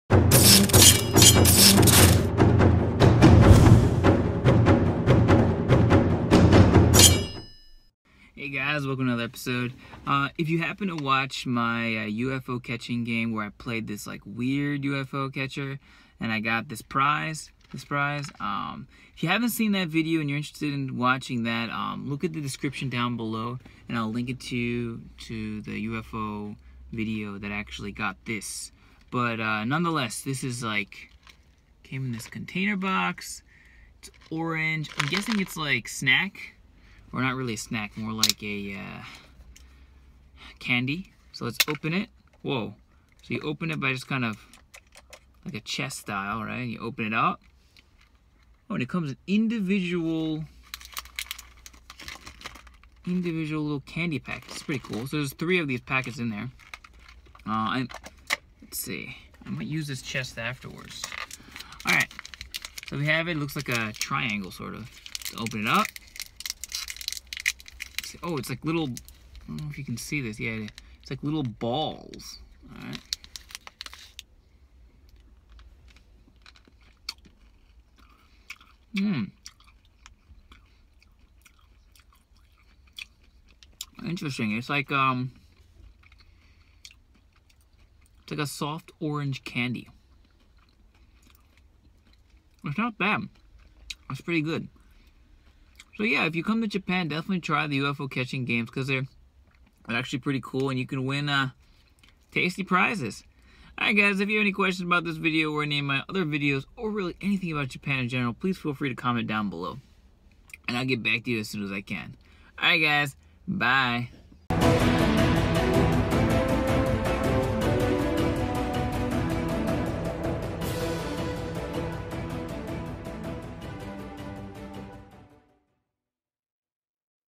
Life in Japan: (Orange Candy Chest!?) E146 (UFO PRIZE)
Welcome to episode 146! In this episode, I review this orange candy prize I fished out of this UFO catcher game! If you have any questions / comments, please write below! Thanks!